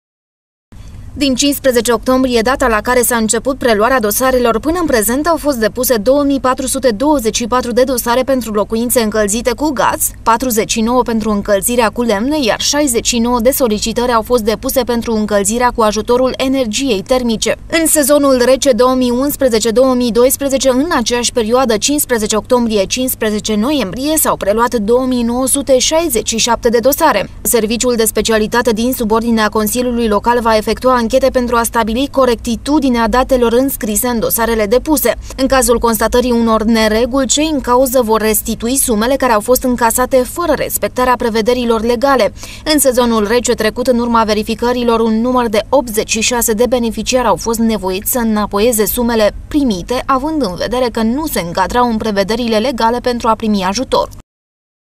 Ajutoare pentru mii de sibieni.mpg
Din 15 octombrie data la care s-a inceput preluarea dosarelor, pana in prezent au fost depuse 2424 de dosare pentru locuinte incalzite cu gaz, 49 pentru incalzirea cu lemne, iar 69 de solicitari au fost depuse pentru incalzirea cu ajutorul energiei termice.